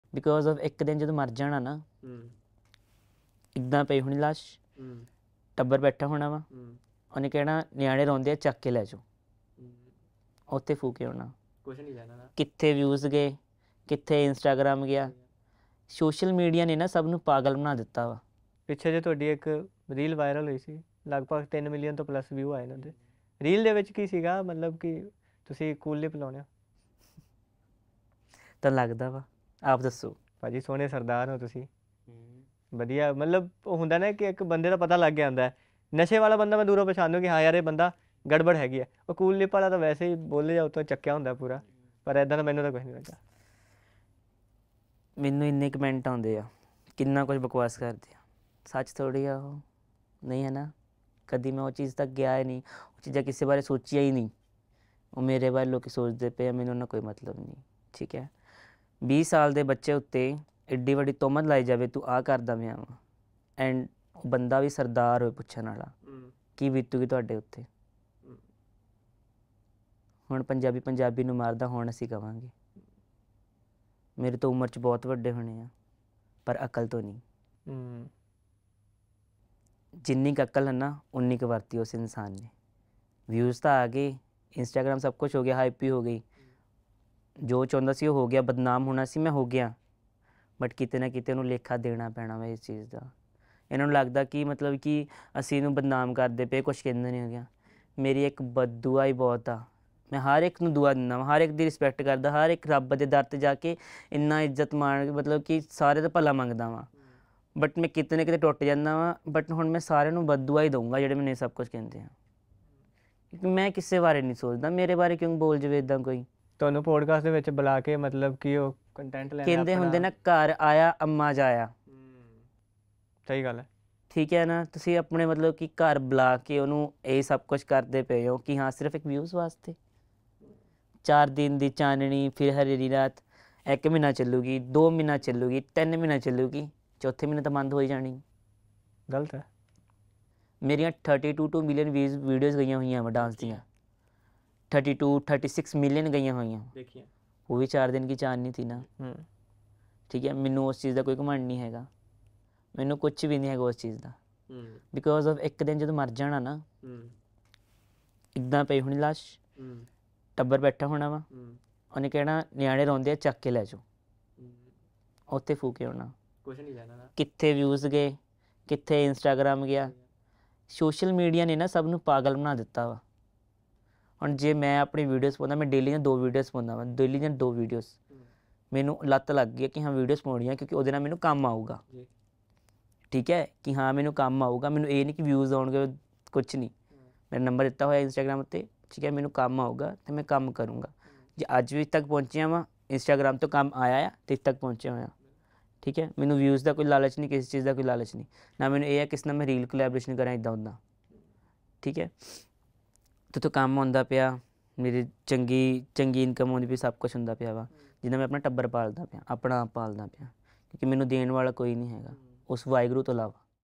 Ballu Ramgaria ਦੀ Cool Lip ਵਾਲੀ Controversy ਪਿੱਛੇ ਕੀ ਸੀ ਕਾਰਨ | Latest Podcast | PTC Punjab
Ballu Ramgaria ਦੀ Cool Lip ਵਾਲੀ Controversy ਪਿੱਛੇ ਕੀ ਸੀ ਕਾਰਨ !

Join Ballu Ramgarhia on the PTC Podcast as he delves into the controversy surrounding Cool Lip. Don't miss this exclusive Podcast on PTC Punjabi's official YouTube channel, where the renowned choreographer and influencer shares his side of the story.